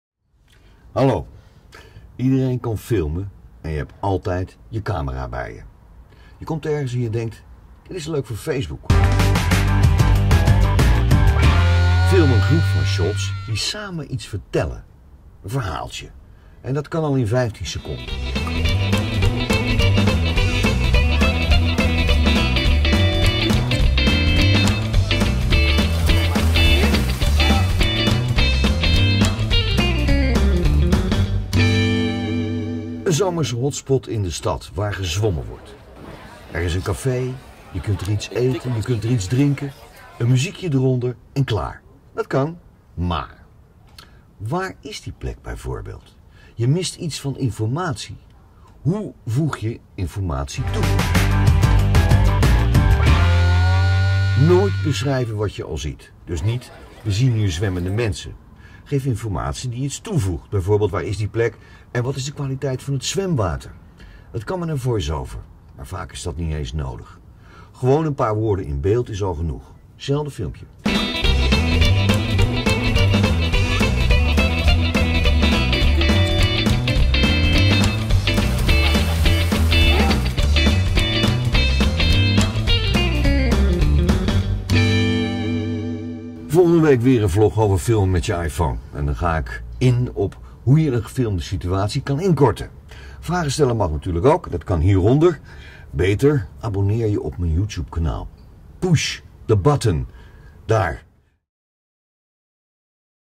Tips Filmen met je Iphone (1)informatie toevoegen
Maak van je shots een kort verhaaltje. Hoe doe je dat?